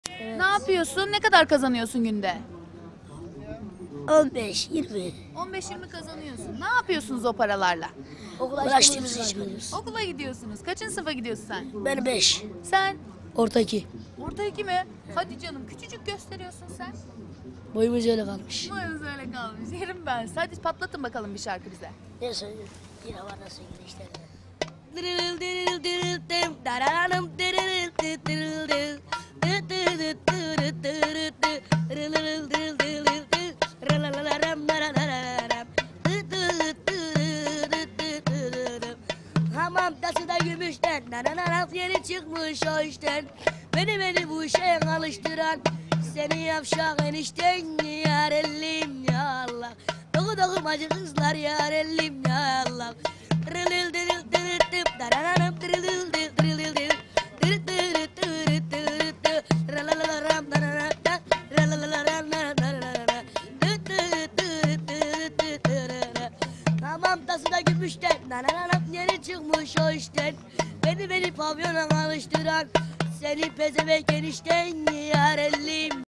Hamam Tası Gümüşten - Eskişehir Sokak Röportajı
ES Tv ana haber muhabirliği yaptığım dönemde, kameraman arkadaşım Güven Duysak ile birlikte Eskişehir Hamam yolu civarında gerçekleştirdiğimiz 4 Ocak Salı ,2011 tarihli sokak röportajımdır.